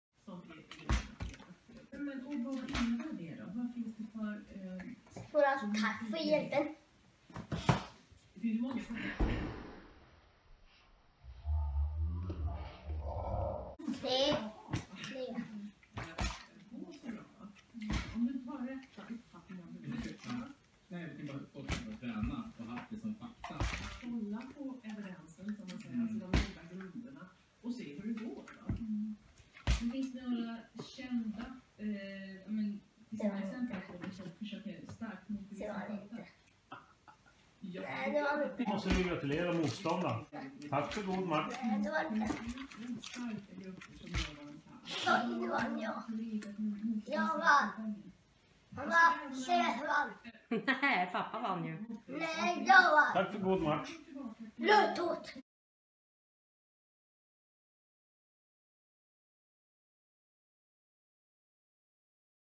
Max flippar flaska & är dålig förlorare
Här ska ni få se hur dålig förlorare Max är 🙊i söndags satt Max och pappa och "flippa" flaska på soffan och oftast brukar Max vinna detta, men denna match gjorde han inte det ☹️😠 kolla in minen då han förstår att pappa vunnit. Det slutade med ett "rött kort" och en utvisning av pappa 😂😉